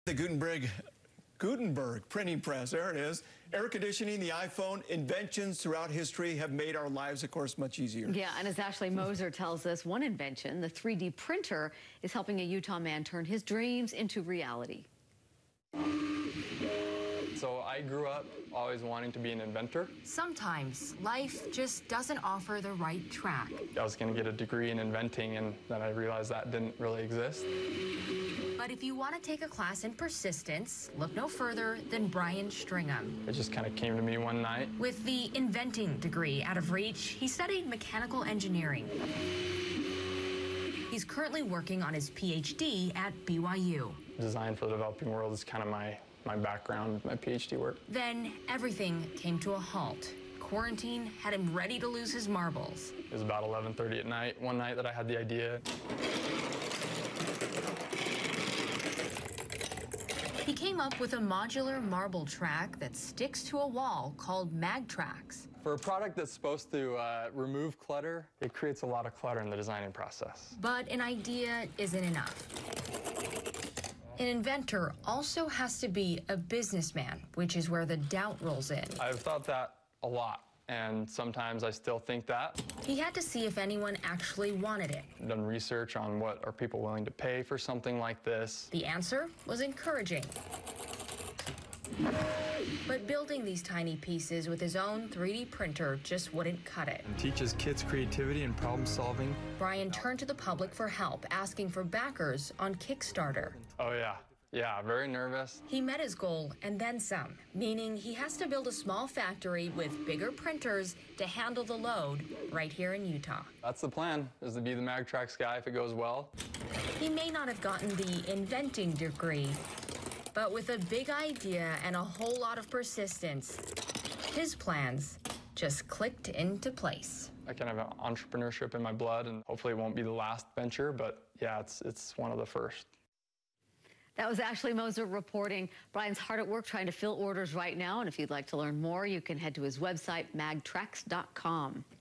BYU PhD Student Turns Inventor With Magnetic Marble Track
Sometimes, life just doesn't offer the right track. With the help of a 3D printer, one Utah man is building his own called "MagTrax."